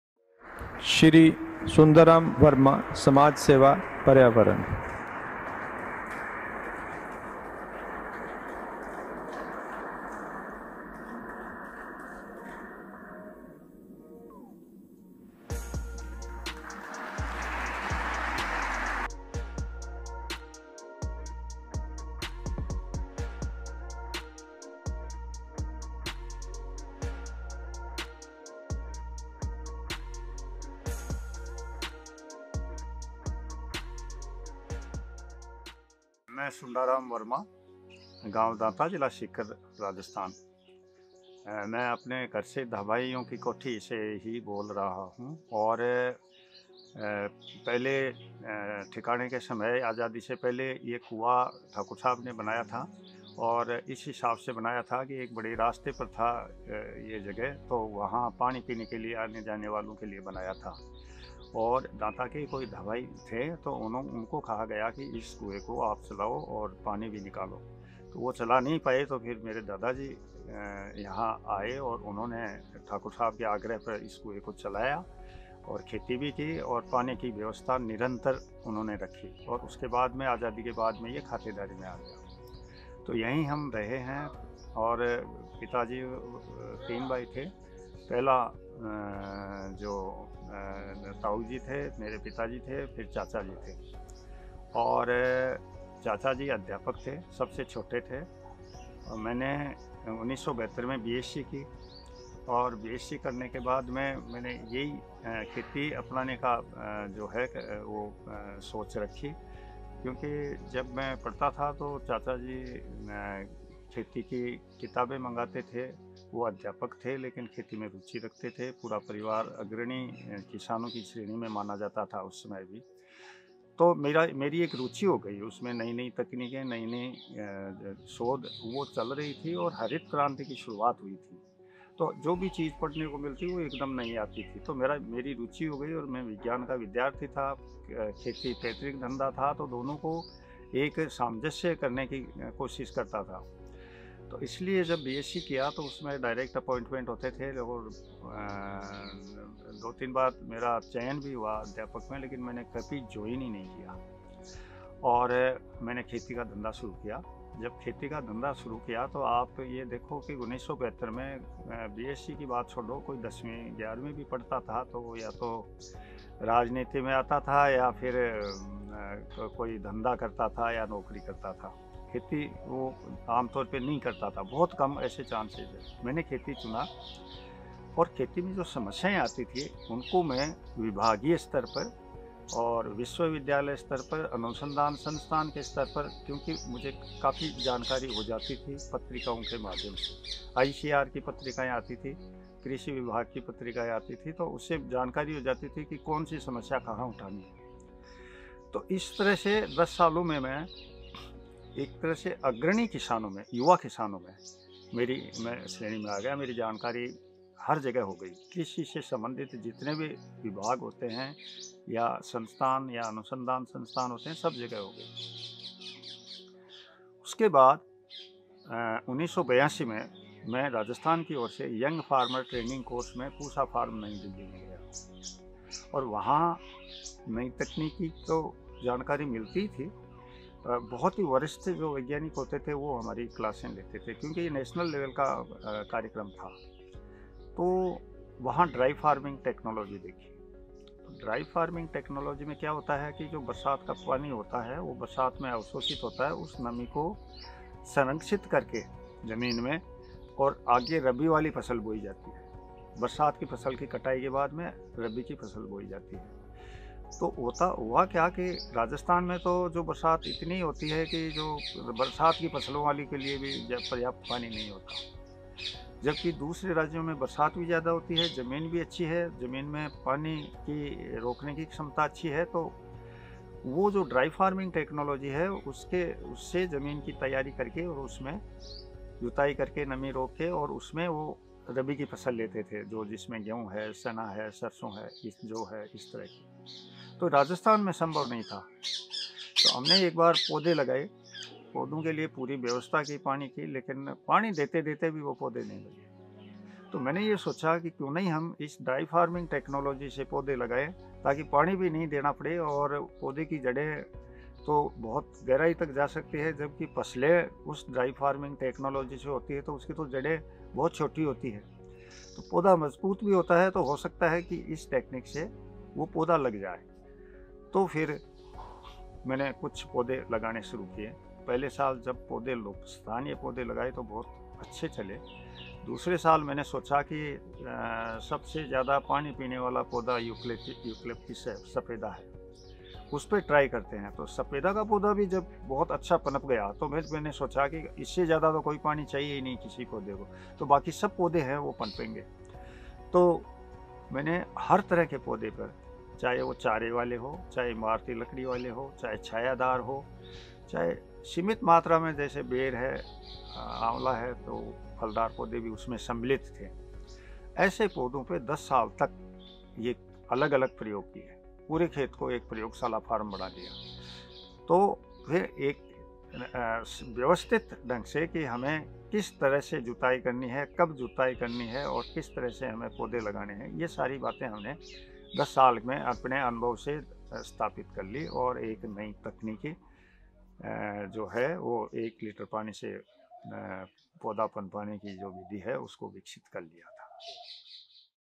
एक इंसान जिसने रेगिस्तान को हरा भरा किया | पद्मश्री सुंडाराम वर्मा
हाल ही में कृषि वैज्ञानिक " सुंडाराम वर्मा  " को राष्ट्रपति भवन मे भारत के राष्ट्रपति श्री रामनाथ कोविंद के द्वारा " पद्मश्री " पुरस्कार से सम्मानित किया गया।

राजस्थान के सीकर जिले दातारामगढ़ तहसील से पद्मश्री सुंडाराम वर्मा ने खोजी 1 लीटर पानी से पौधा लगाने की तकनीक। इस अनोखी तकनीक के द्वारा आने वाले समय में अब पानी की निर्भरता को कम करके सिंचाई पूर्णतया कर सकते हैं।

इन्होंने सन 1972 में अपनी एग्रीकल्चर बीएससी की शिक्षा पूर्ण की तथा इनको यह शिक्षा किताबों के साथ साथ परिवार के बड़े बुजुर्गों से मिली जो कृषि में रुचि रखते थे। इसके बाद कृषि में इतनी रुचि बढ़ गई कि 3 सरकारी नौकरियों को त्याग दिया और हरित क्रांति के चलते उस समय के प्रचलित कृषि से संबंधित ज्ञान प्राप्त करने में लग गये।

खेती में आने वाली समस्याओं को विश्वविद्यालय स्तर पर, कृषि विज्ञान केंद्र स्तर पर तथा ICAR की पत्रिकाओं के माध्यम से उन्होंने पढ़कर, उनको समझकर उनका निवारण किया।

सुंडाराम वर्मा 1982 में राजस्थान की ओर से यंग फार्मर ट्रेनिंग कोर्स के लिए पूसा नई दिल्ली गए। वहां से इनको कृषि की आधुनिक जानकारी प्राप्त हुई तथा राष्ट्रीय स्तर के वैज्ञानिकों से मिलने का मौका मिला। यही से इनको ड्राई फार्मिंग की शिक्षा प्राप्त हुई।

पद्मश्री सुंडाराम वर्मा की 1 लीटर पानी से पौधा लगाने की तकनीक की खोज ने लगभग 50000 से अधिक पौधों को जीवनदान दिया है जो कि पर्यावरण को पूर्णतया सुरक्षित रखते हैं।
सुंडा राम वर्मा के इस अनोखे प्रयास ने कृषि वैज्ञानिकों को भी अचंभित किया तथा इनके इस अनोखे प्रयास ने कृषि में होने वाली असुविधा को पूर्णतया खत्म कर दिया।

Tags -
Padmashri Sundaram Verma
Farmer Sundaram Verma from Sikar Rajasthan
Environmentalist Sundaram Verma